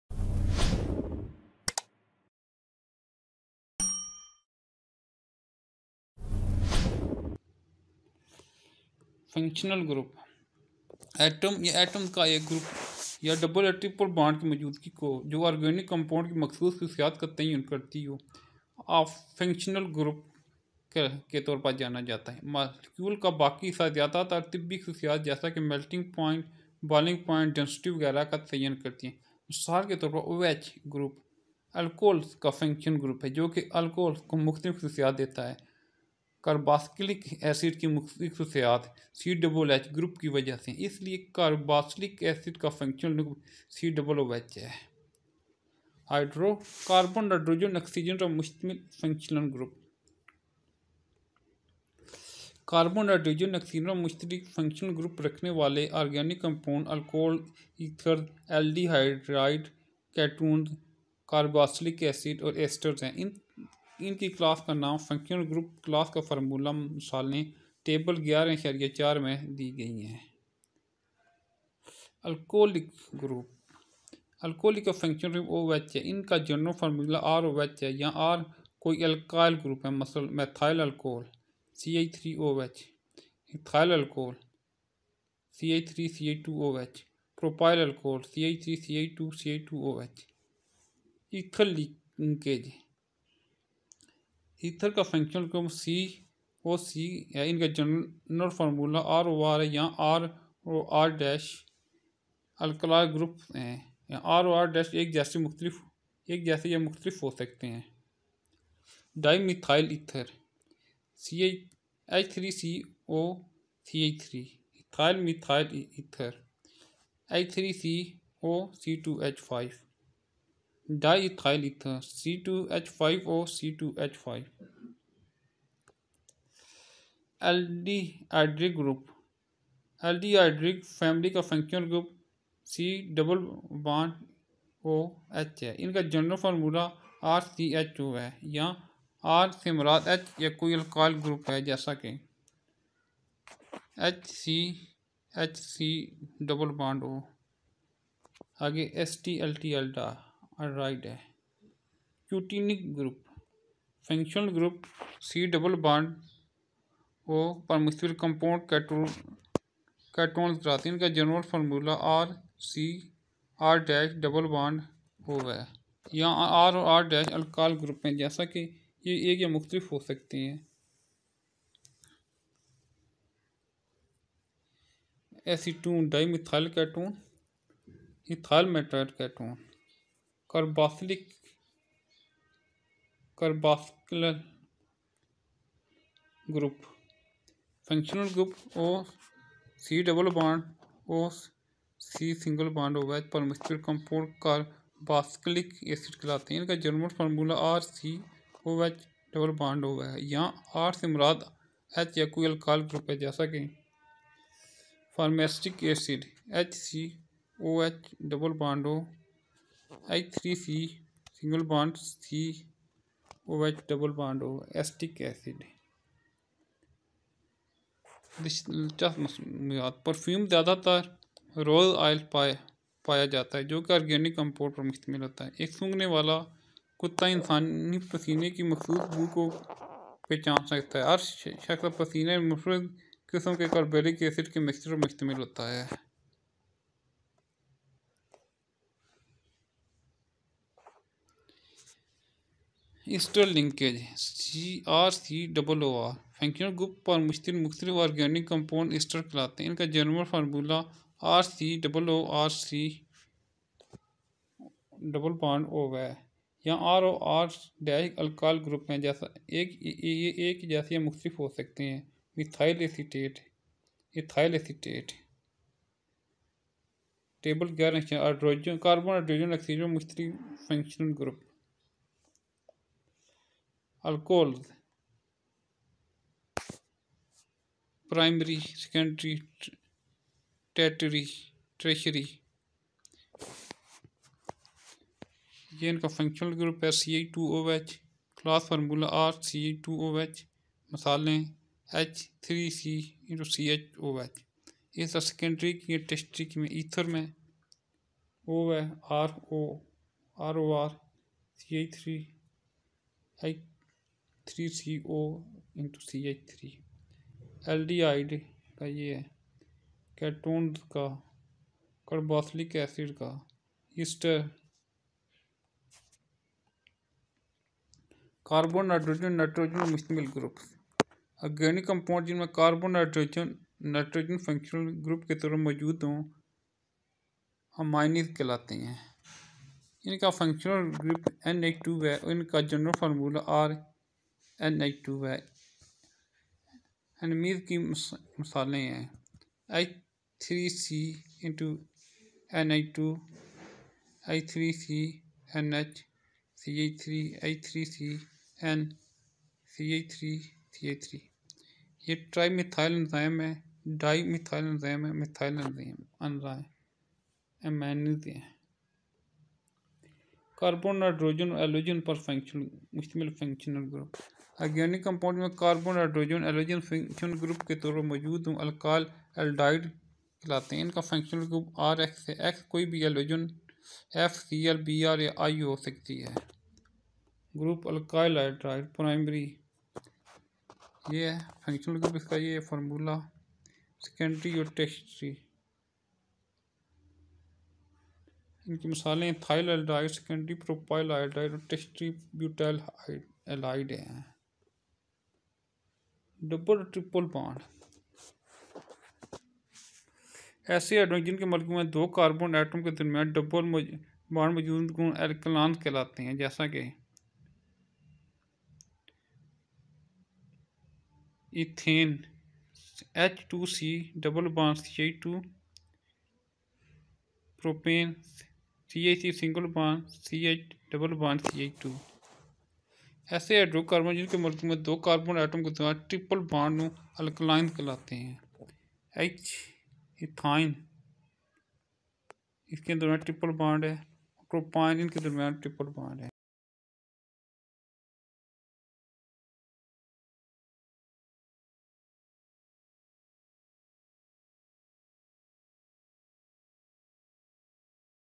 Functional Groups in Urdu Class 10 Chemistry Chapter 11
Functional Groups, Functional Groups Containing Carbon, Hydrogen and Oxygen, Functional Group Containing Carbon, Hydrogen and Nitrogen, Functional Group Containing Carbon, Hydrogen and Halogens,Double and Triple Bond in Urdu Class 10 Chemistry chapter 11